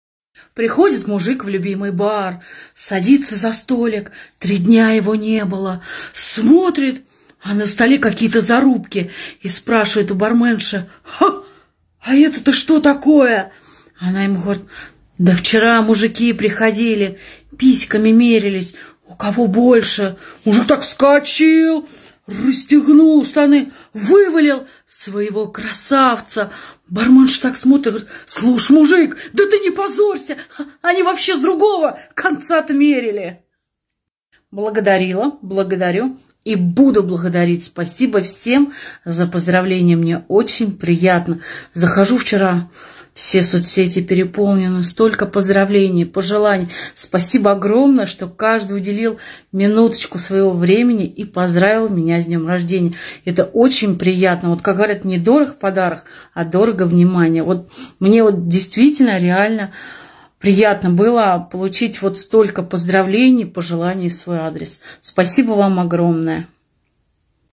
Анекдот показал барменше свой штырёк
Анекдот показал барменше свой штырёк. Приходит мужик в ресторан в котором не был 3 дня, смотрит а на столе зарубки, барменша ему говорит это мужики меряли свои штырьки. Достал он свой померял, а барменша его за смущала мол не с той стороны меряешь.

► Ссылка на плейлист (Юмор Анекдоты).

Смешной и короткий получился анекдот как мужик показал барменше свой штырёк. Мужик снял штаны и решил померять свой штырёк, но увы его разочаровала барменша, сказав что с другой стороны нужно мерять.

► Ссылка на это видео (Анекдот показал барменше свой штырёк).